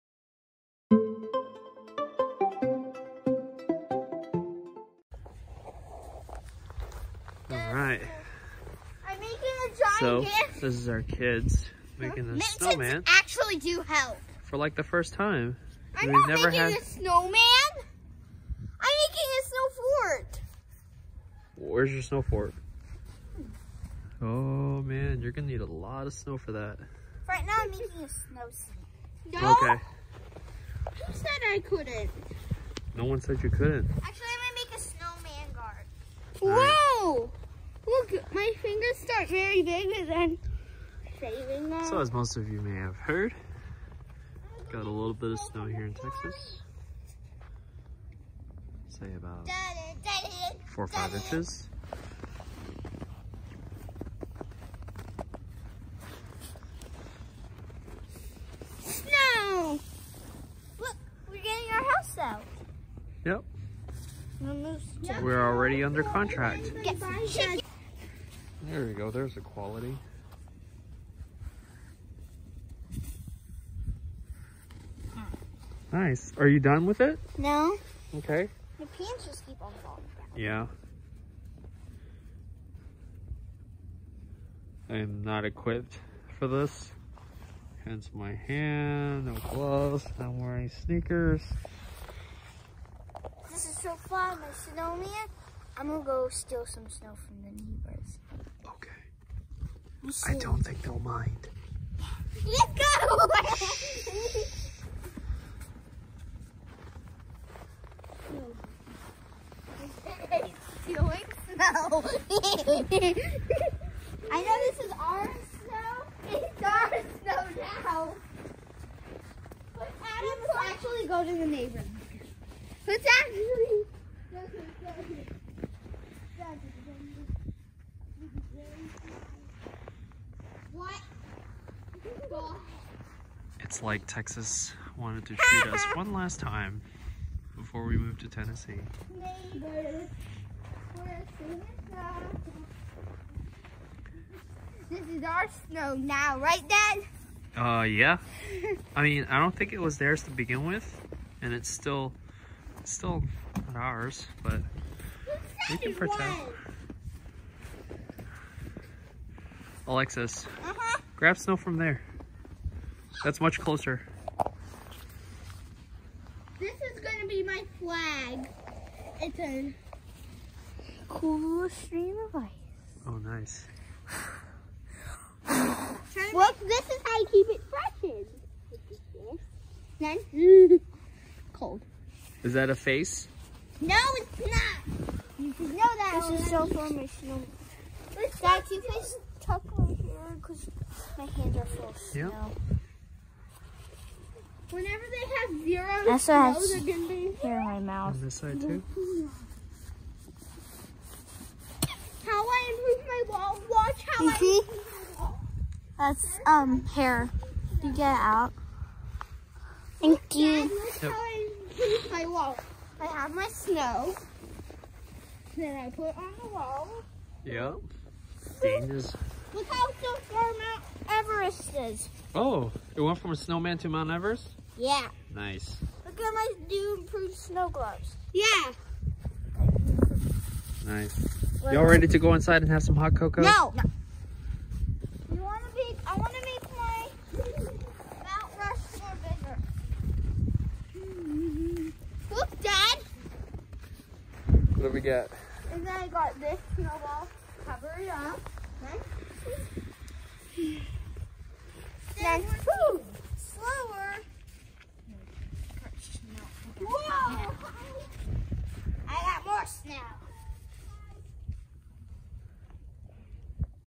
Worst snow storm in Texas 2021
Our kids experiencing a "real" snow fall for the first time! More videos to come - our home update and selling experience and more!

The Lee Family